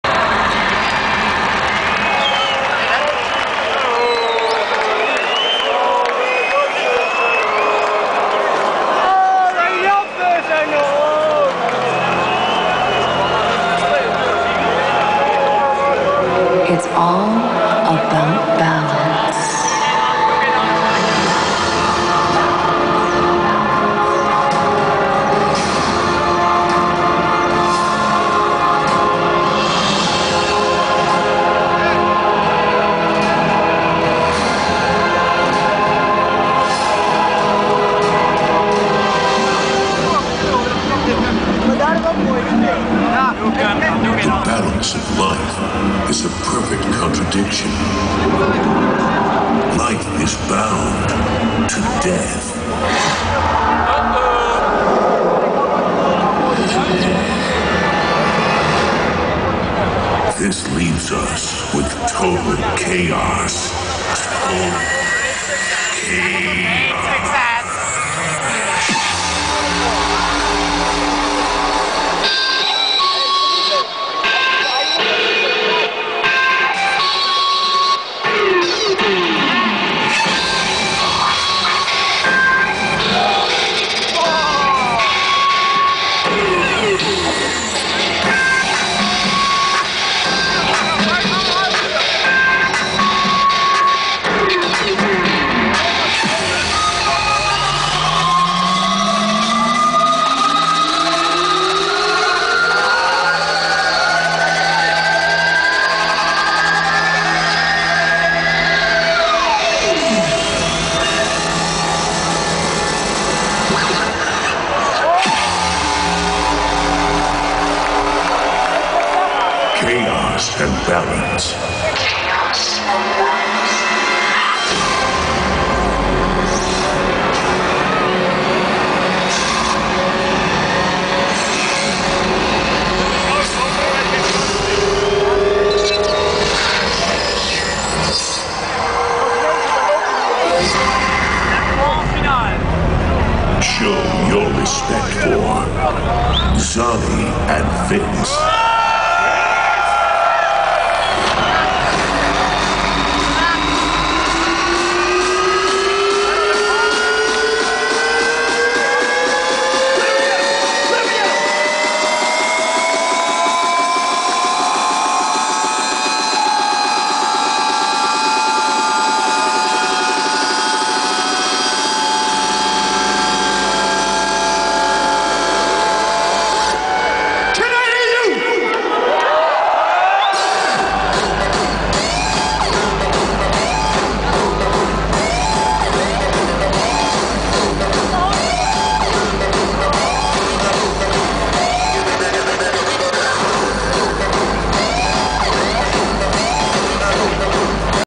Intro Zany & Vince @ Qlimax 2008
The Intro of DJ Zany and Dj Vince at Qlimax 2008